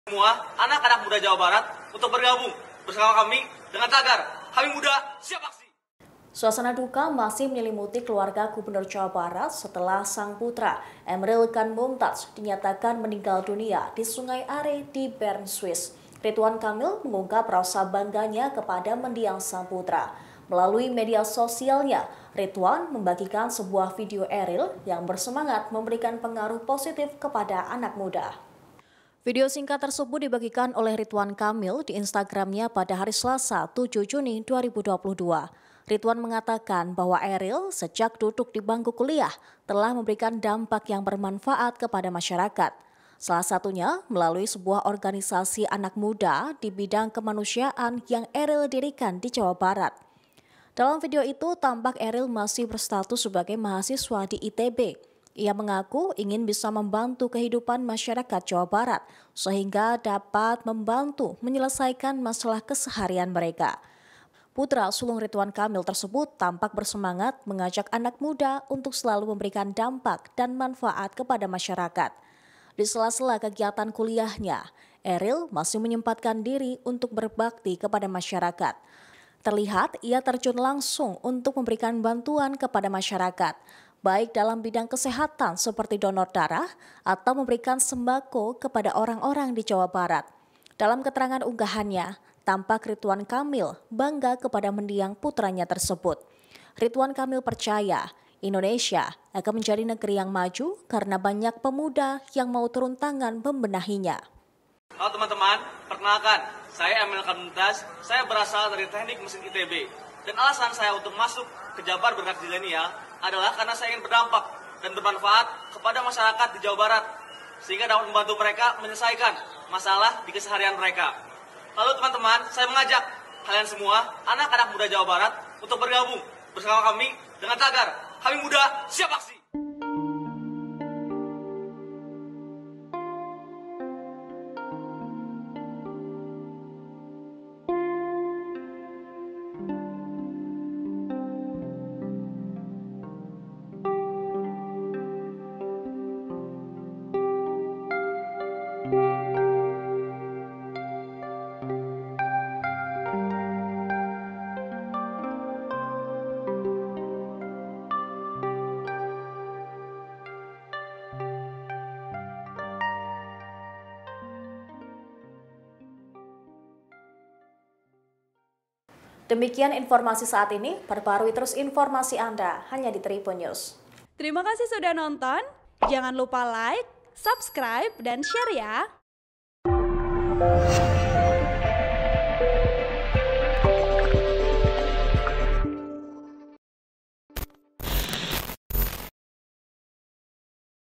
Ridwan Kamil Unggah Video Eril, Ungkap Rasa Bangga pada Mendiang sang Putra: Itulah Semangat Eril
TRIBUN-VIDEO.COM - Suasana duka masih menyelimuti keluarga Gubernur Jawa Barat setelah sang putra Emmeril Khan Mumtadz dinyatakan meninggal dunia di sungai Aare, Bern, Swiss.

Ridwan Kamil mengungkap rasa bangganya kepada mendiang sang putra,

Melalui media sosialnya, Ridwan Kamil membagikan sebuah video Eril yang bersemangat memberikan pengaruh positif kepada anak muda.

Host Saradita